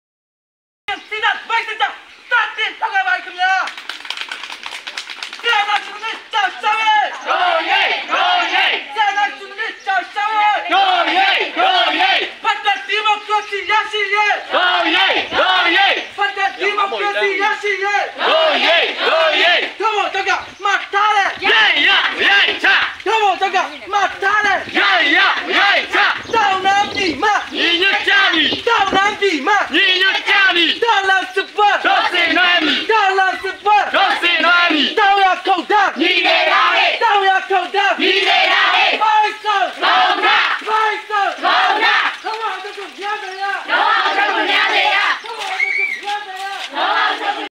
အကြမ်းဖက်စစ်အာဏာရှင် ချေမှုန်းရေး ဆားလင်းကြီးမြို့နယ်သပိတ်စစ်ကြောင်း
အကြမ်းဖက် ဖက်ဆစ် စစ်အာဏာရှင် ဆန့်ကျင်ရေး ဆားလင်းကြီးမြို့နယ်သပိတ်စစ်ကြောင်း ဟာ ယနေ့ မေလ ၁၃ ရက်တွင် လှည့်လည်ကြွေးကြော်ဆန္ဒပြကြသည်။